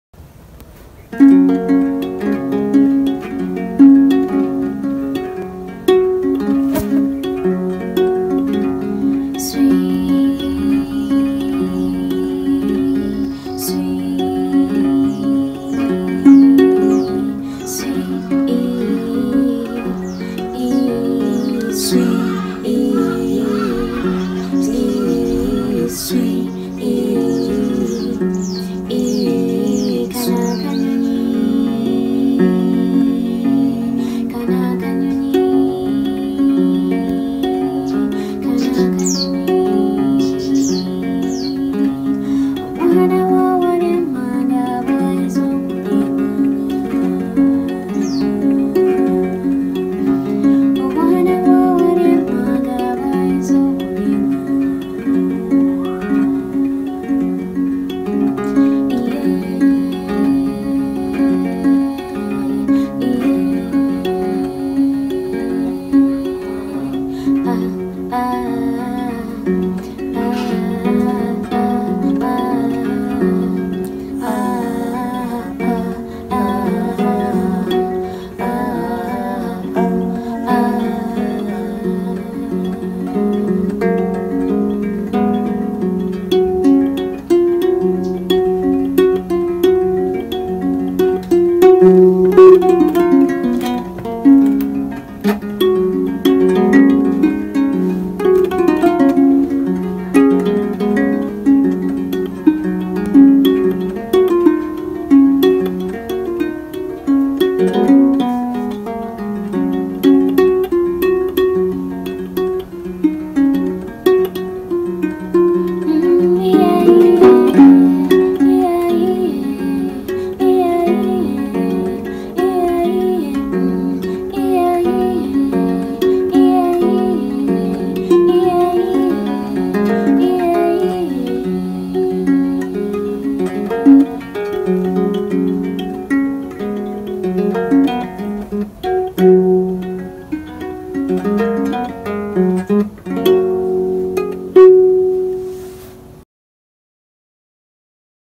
Fast fingering motifs on Adungu ( Bow harp)
A duo you can play on Adungu (bow harp)